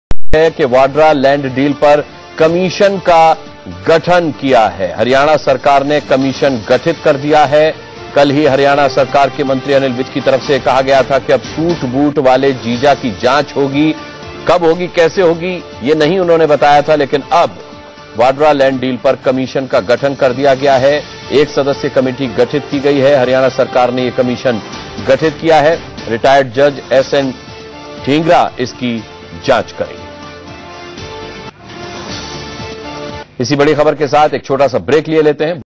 Haryana govt forms commission to probe Robert Vadra land deals
Haryana government on Thursday formed a one-man commission to investigate land deals of Robert Vadra during the previous Congress regime in the state.Retired Justice from Delhi High Court has been appointed as chairman of the newly formed commission.